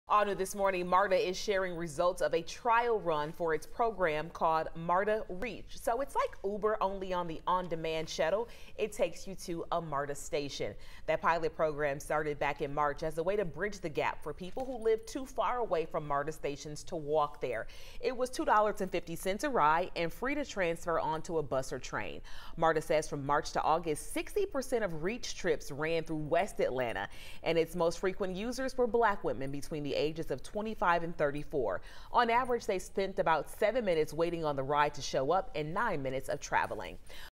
MARTA Reach program serves thousands in trial run
It's like Uber, only the shuttle takes you to a MARTA station.